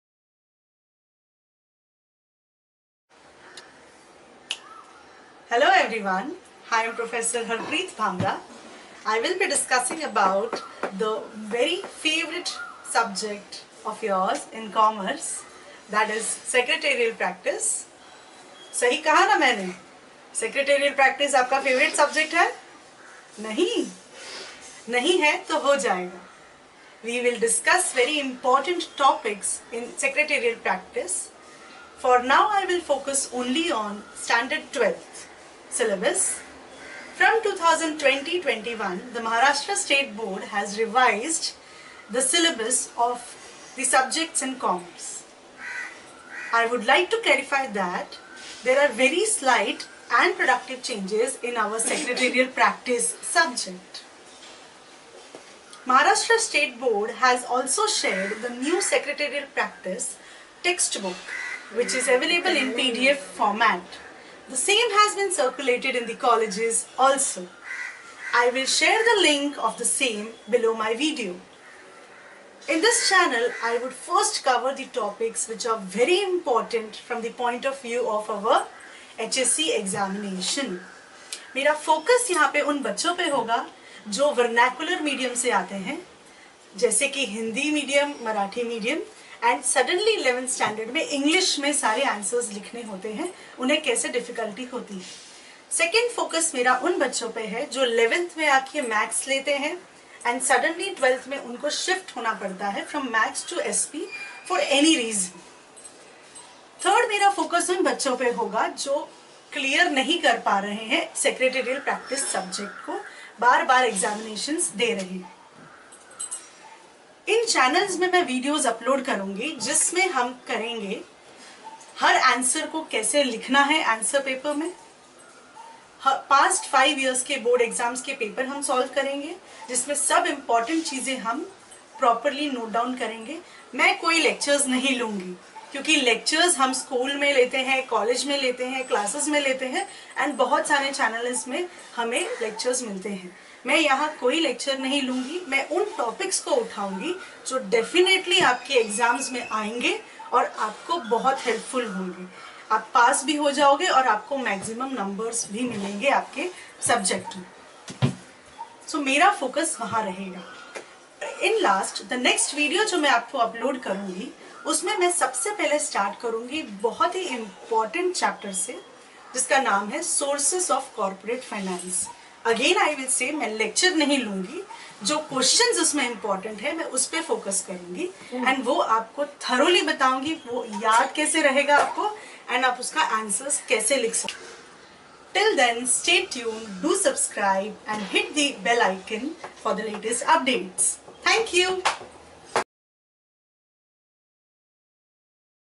Secretarial Practice Introduction | 12th Commerce | Explained in Hindi
Introduction to Secretarial Practice Maharashtra Board 12th Standard Commerce New syllabus 2020-21

Secretarial Practice Introduction | 12th Standard | SP New Syllabus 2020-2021